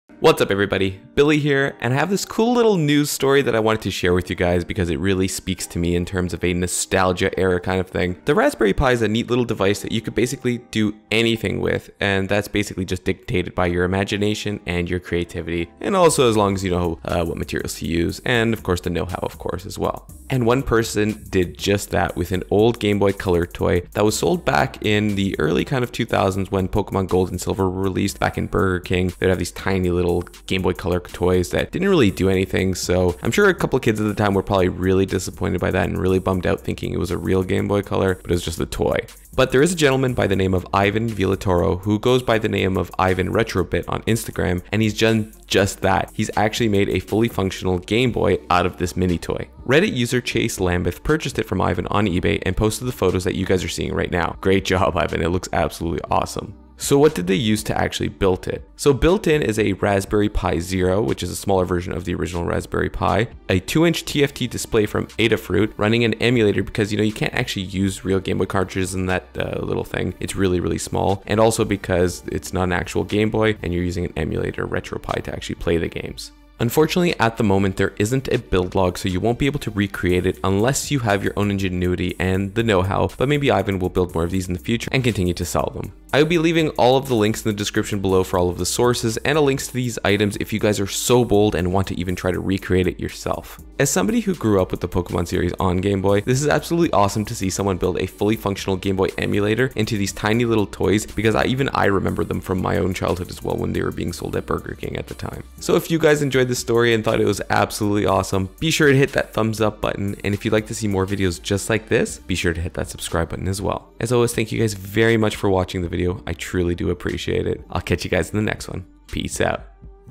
WORLD'S SMALLEST GAME BOY! (Raspberry Pi Zero News)
Ivanretrobit created a tiny Game Boy emulator with a Raspberry Pi!

Enjoy gaming videos?! Check out the gaming channel!